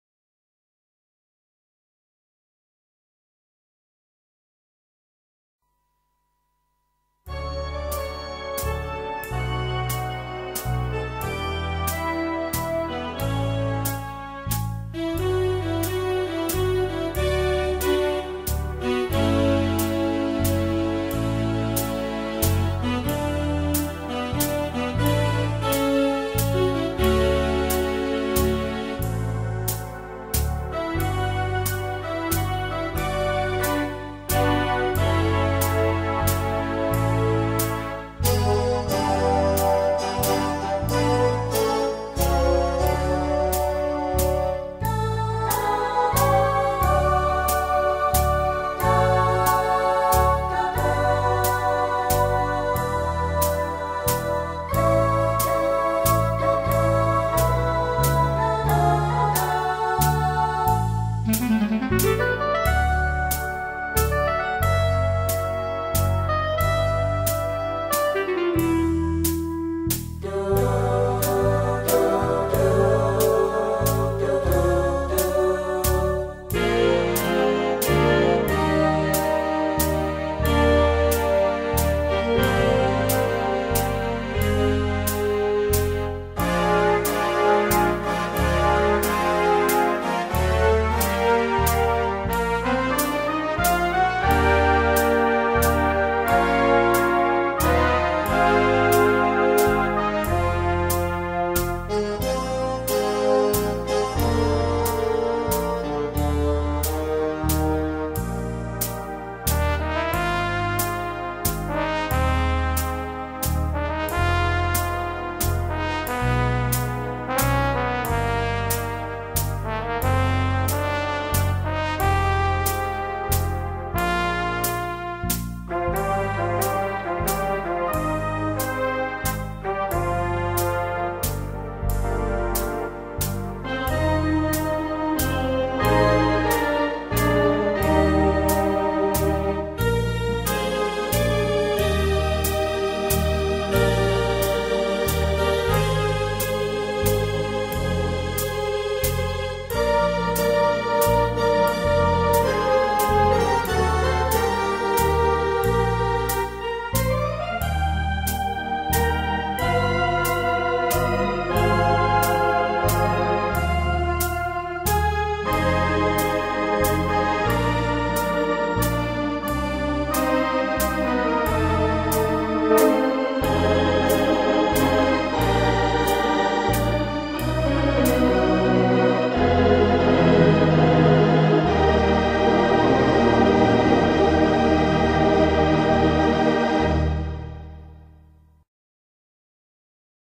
Demo by Karl on TTT V3 Ultra-plus midi module =G.Miller type sounds
Another Demo by Karl,
this time playing some of our G.Miller Bigband sounds.
He adds style / drums from the AT80 on this one.

note ; at around 2.14 He uses our Miller + Tower settings,
ending with full Tower